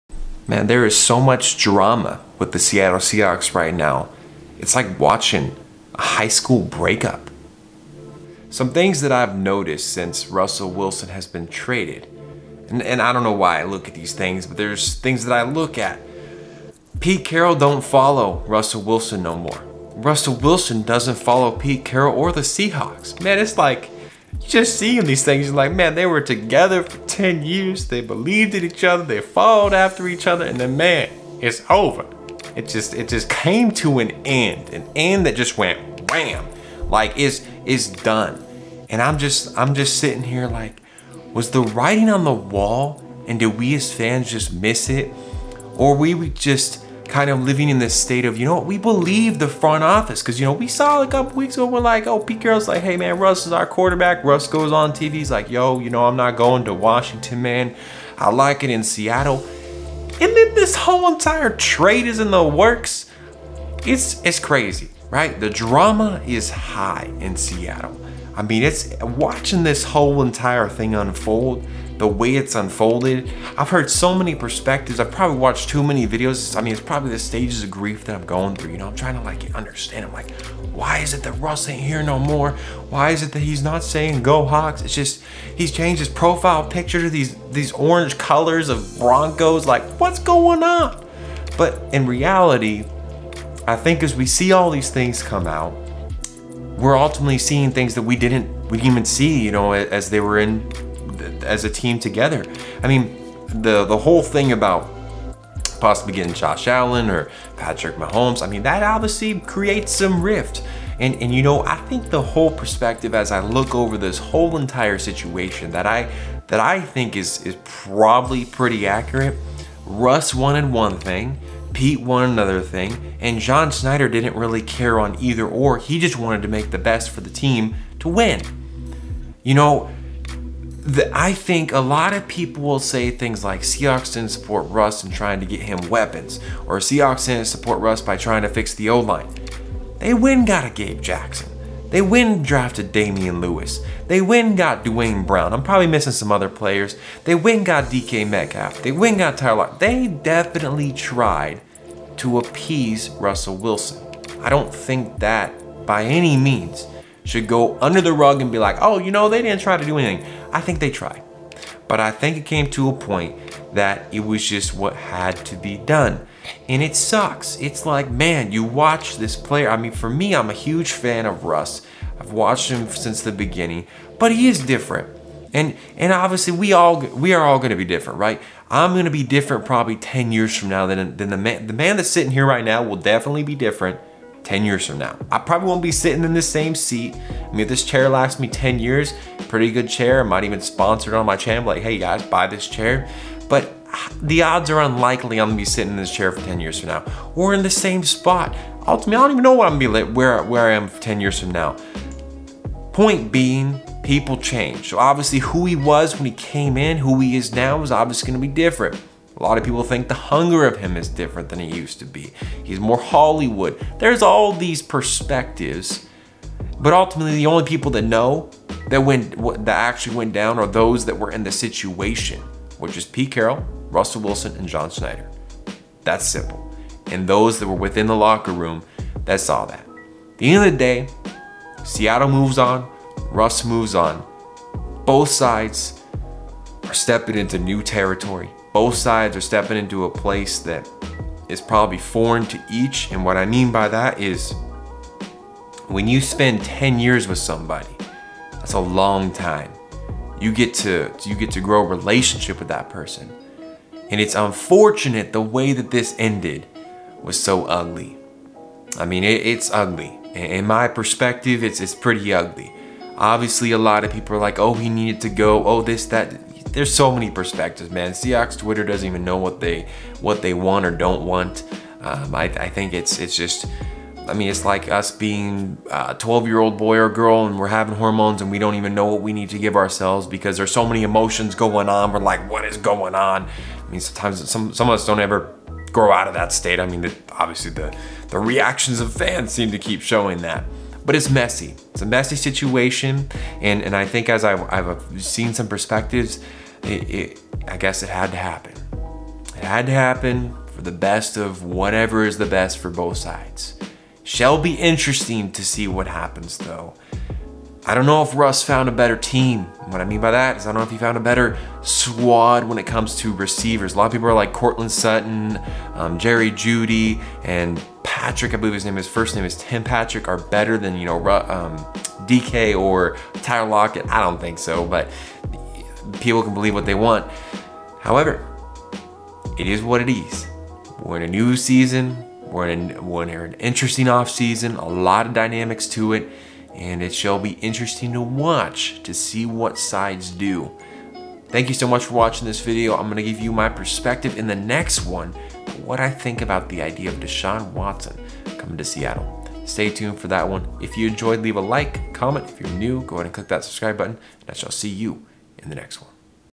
Russell Wilson and Pete Carroll: The End of an Era
Russell Wilson no longer follows Pete Carroll? Pete no longer follows Russell Wilson? Who you follow on social media isn't a big determination on whether or not you like someone but the end of the era has many questions. I explore some of those questions today and give my response.